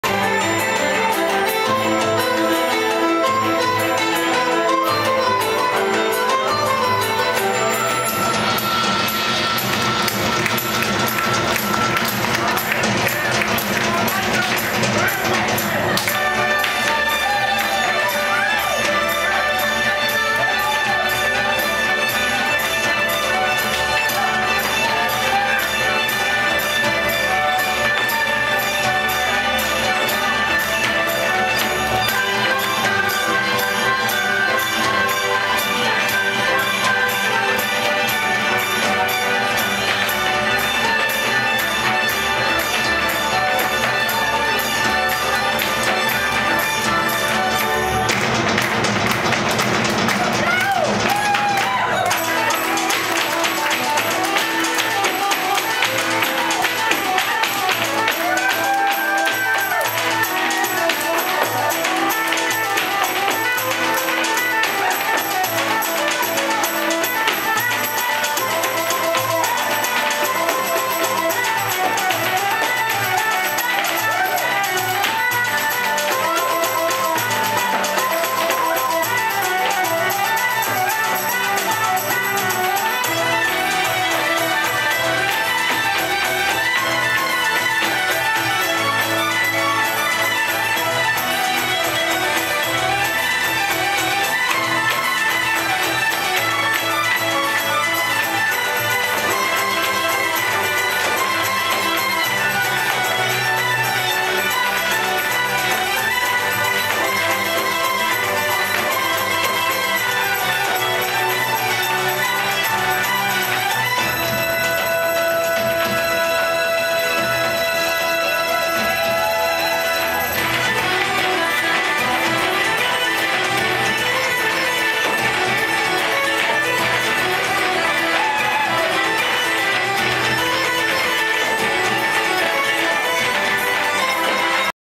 Irish Dancing
On our last night, Isobelle and I went to this Irish Dancing/Singing dinner show thing. Quite fun! This was one of the Irish Dancing Scenes.

These videos don't really give the dancers justice. You can't hear it over the audio, but the shoe taps match the beat perfectly, at the dancers do this as well as the most intricate moves! Like in the space of a second, a heel tap & double tap & heel tap combination. Incredible.